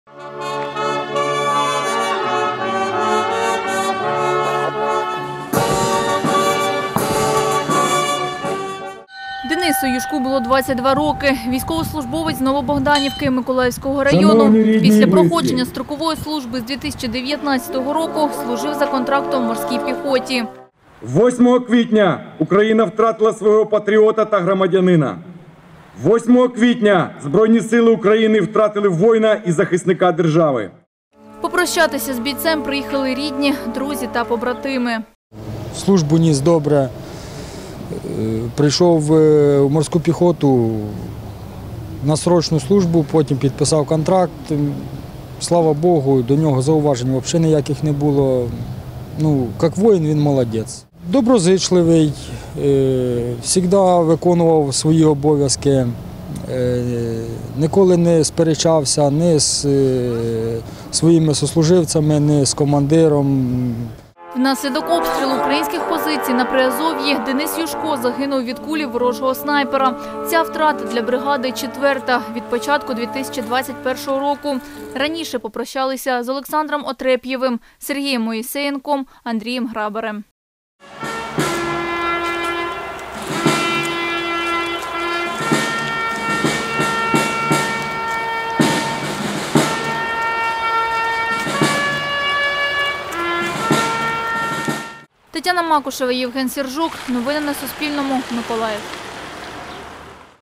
Четверта втрата від початку року. У Миколаєві попрощалися із загиблим матросом 36-ї бригади
12 квітня на плацу військової частини А2802 (36-ї окремої бригада морської піхоти імені контр-адмірала Михайла Білинського) попрощалися з загиблим військовослужбовцем Денисом Юшком. 22-річний військовий загинув 8-го квітня від кулі ворожого снайпера на Приазов’ї.

Авторка: Тетяна Макушева, оператор: Євген Сержук.